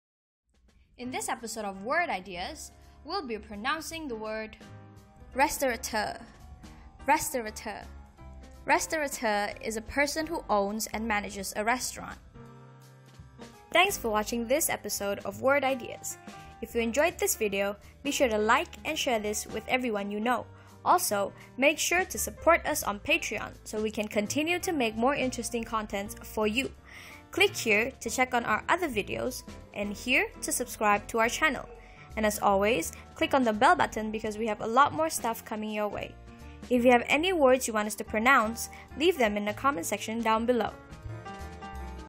How to pronounce Restaurateur| How to say Restaurateur | What is Restaurateur
How to pronounce Restaurateur? in today's episode, we'll find out how to pronounce it as well as learning the meaning of it as well.

The meaning of Restaurateur is: A person who owns and manages a restaurant.